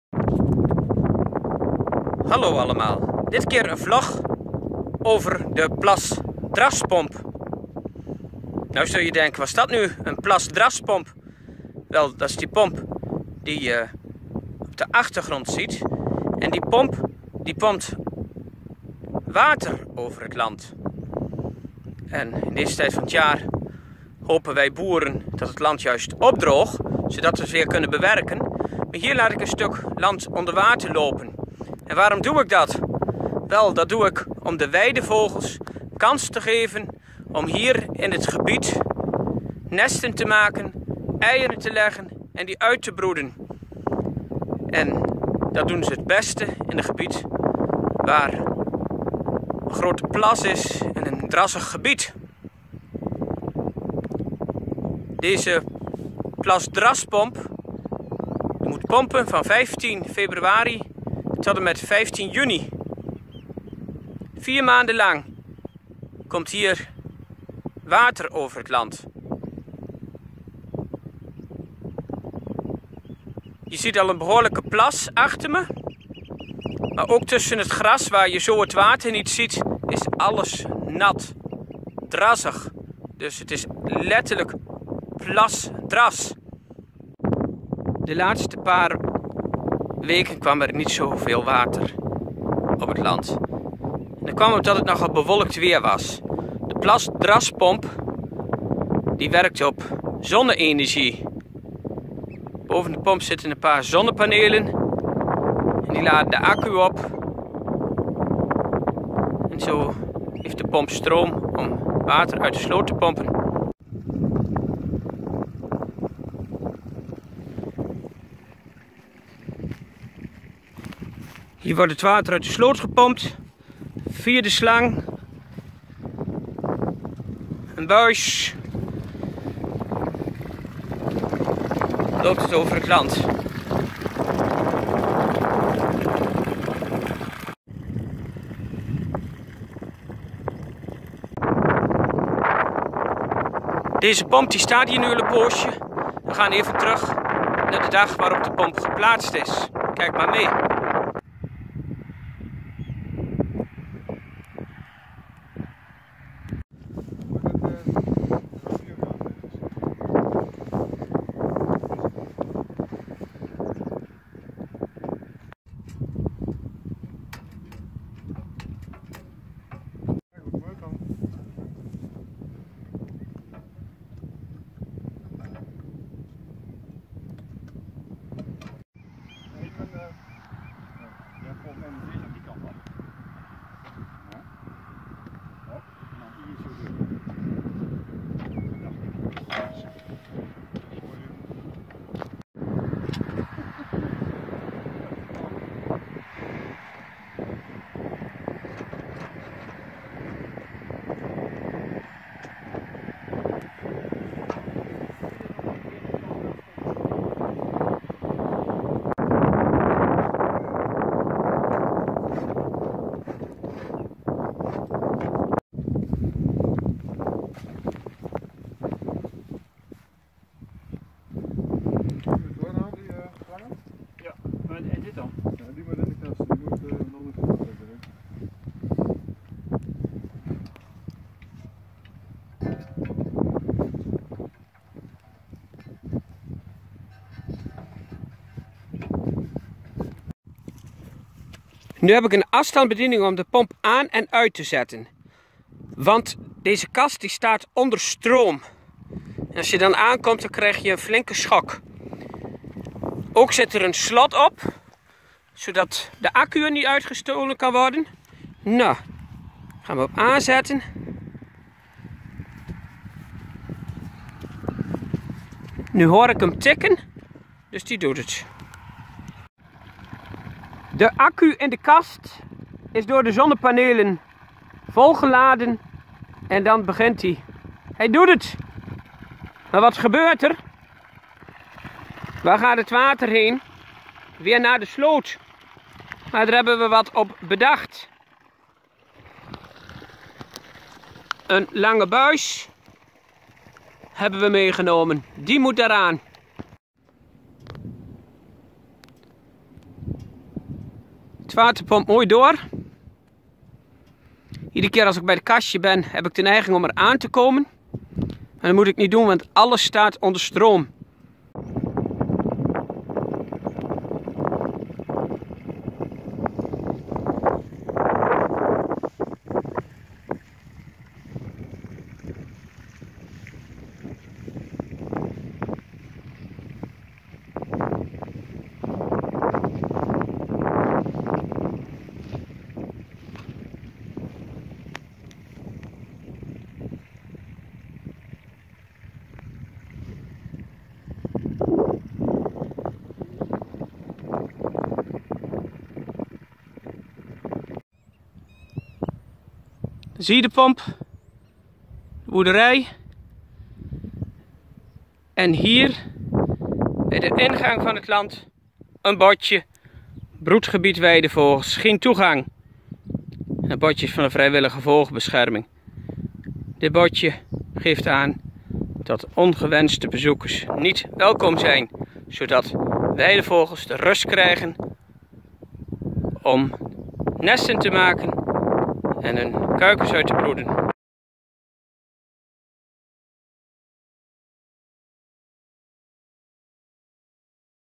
Boer Gerrit vlogt - Plasdras-pomp redt de weidevogels
Om het voortbestaan van de weidevogels te redden wordt groot materiaal ingezet.